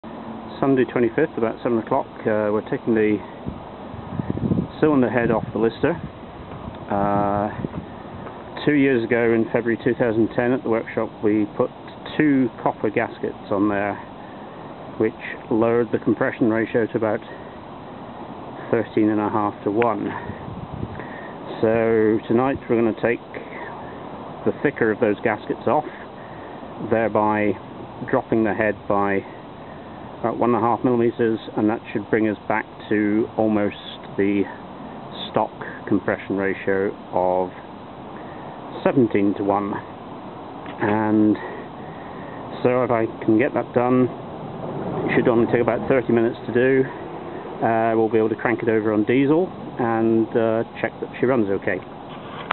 Lister cylinder head removed
Lister cylinder head removed in preparation for removing additional gaskets. This is to restore the compression ratio to around 17:1.